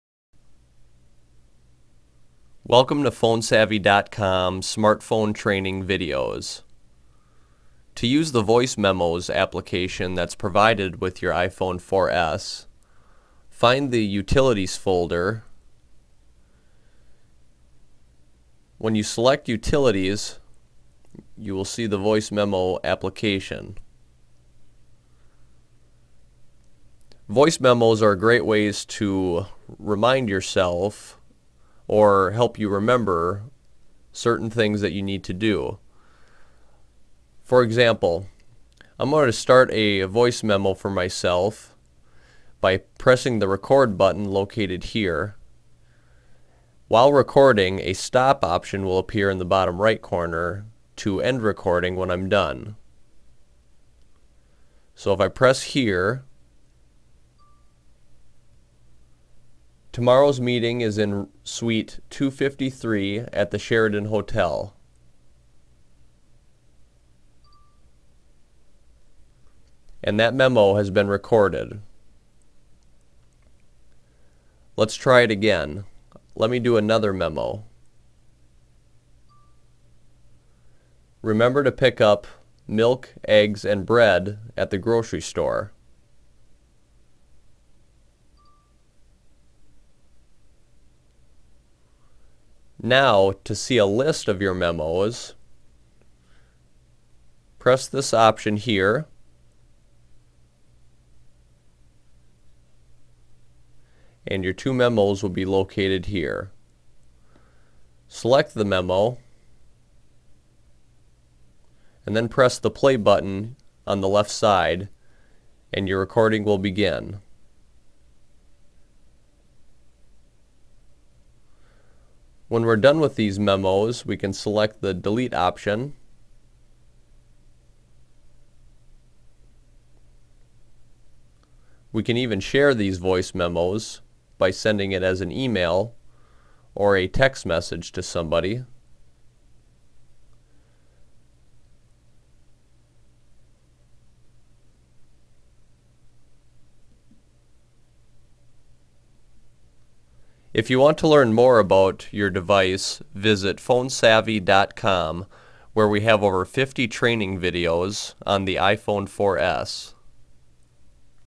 How Do I Use The Voice Memos App On My Apple iPhone 4S?
This video will show you how to use the "Voice Memos" app on the iPhone 4S.